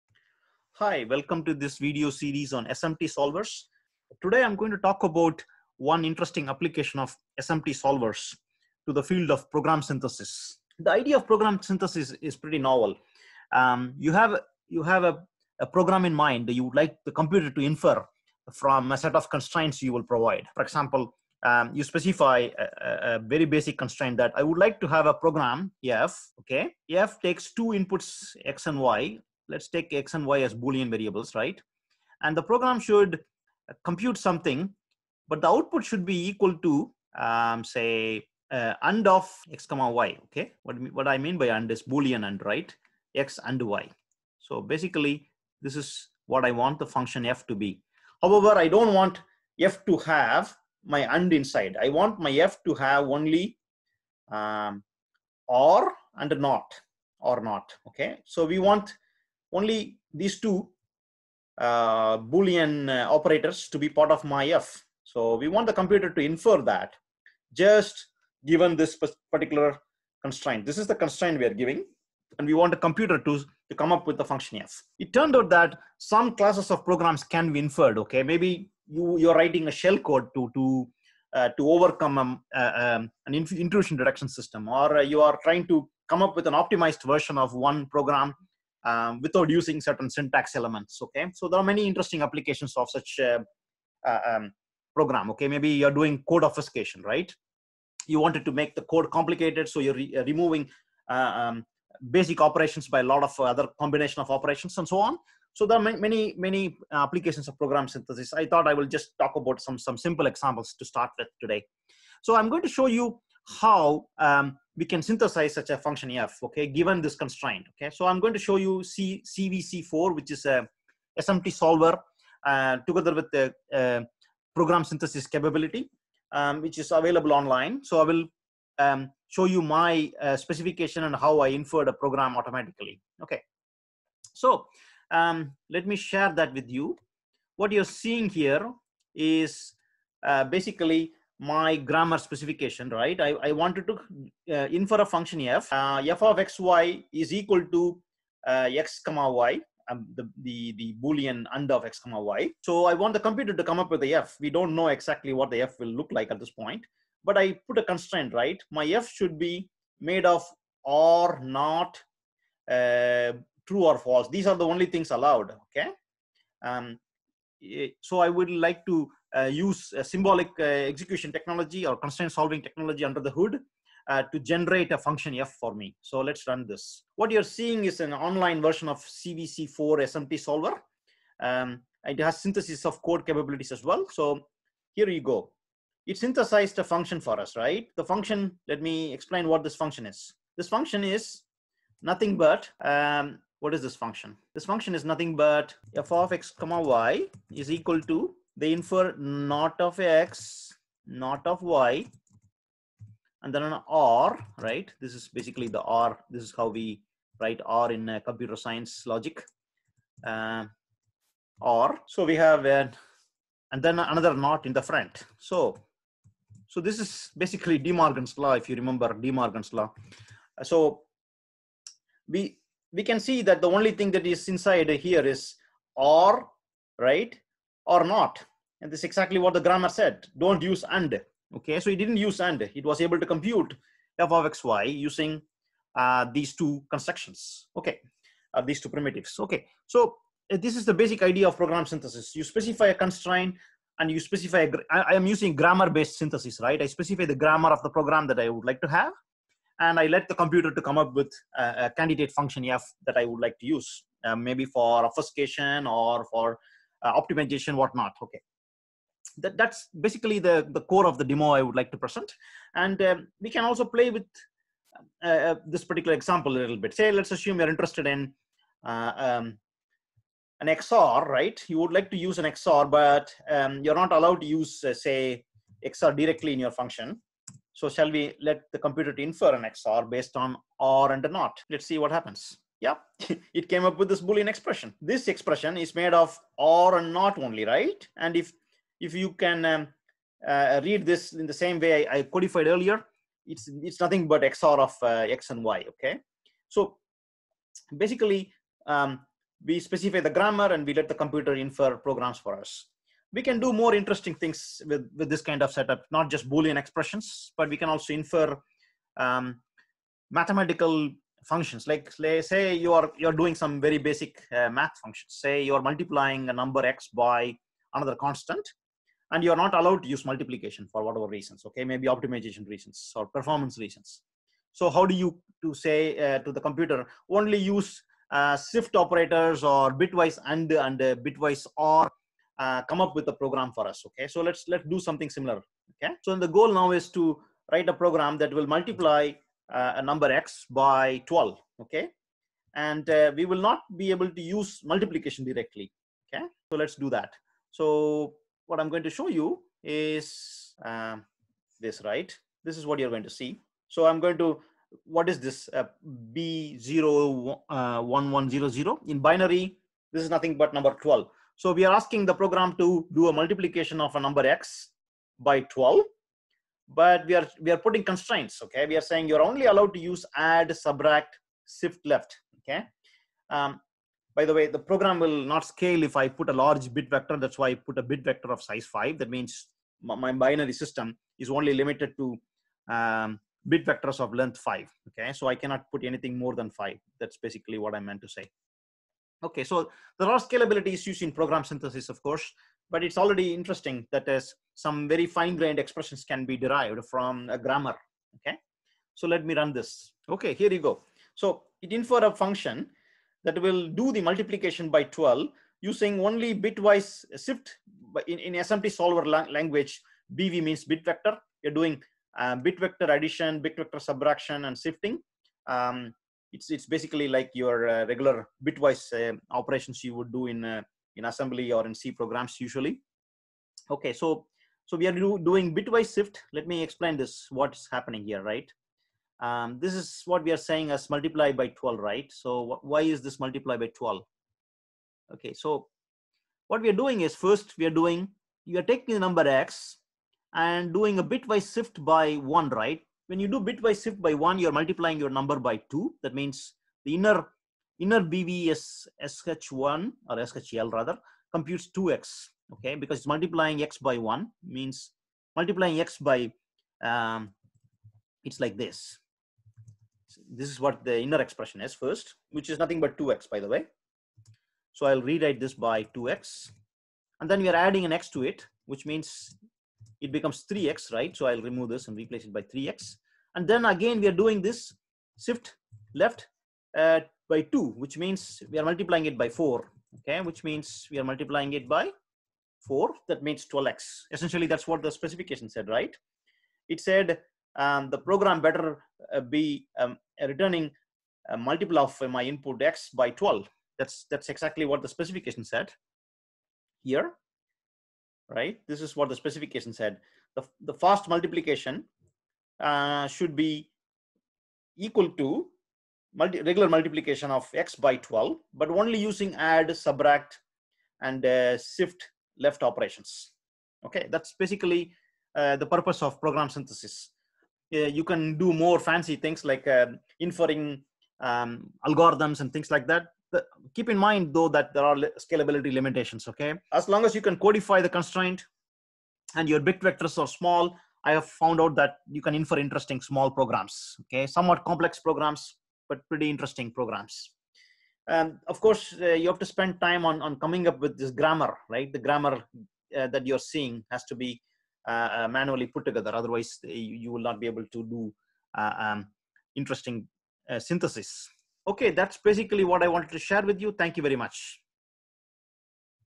Demo: Program Synthesis using SMT solvers
We present a few basic examples of how to synthesis programs from specifications.